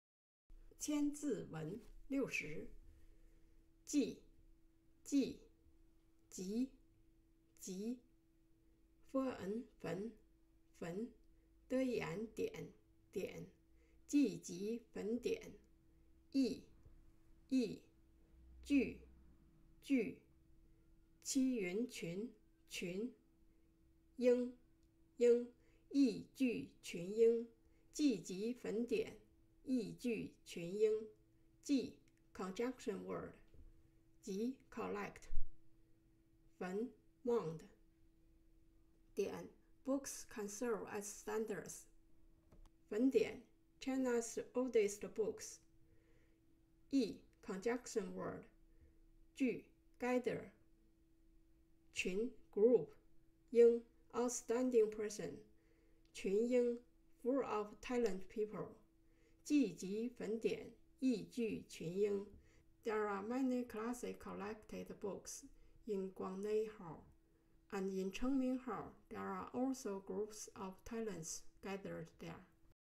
千字文60-qianziwen@Helen Chinese-Thousand words ,既(jì)集(jí)坟(fén)典(diǎn)，亦(yì)聚(jù)群(qún)英(yīng)
The original main purpose of "The Thousand Character Essay" is for learning to read, but due to Zhou Xingsi's excellent literary talent, he compiled unrelated words into an exquisite article. These thousand single un-repeated characters combined as some preface chants all-encompassing knowledge about astronomy, natural history, society, history, ethics, education, etc. It has a strict and simple structure, high literary talent, neat dialogues, and smooth rhythms which is admirable. It can not only be used for literacy, book learning, and composition writing, but also can be used to broaden one's knowledge, so it is the first comprehensive reading material in the history of our country.
According to my own learning progress, I try to understand 8 words at a time, and every 40 words will be used as a familiar pronunciation group. In the future, I will use a group of 500 words to achieve the purpose of memorization. I hope friends who are interested in learning Chinese will follow my script and memorize these 1,000 words.
The buildings inside the Chang'an Palace in west capital led to the Guangnei Hall to the right and the Chengming Hall to the left.
and the Guangnei Hall is the place where books are collected, including books written in remote antiquity.
The Chengming Hall is the place where the emperor meets with hundreds of civil and military officials, there are also many celebrities gathered there.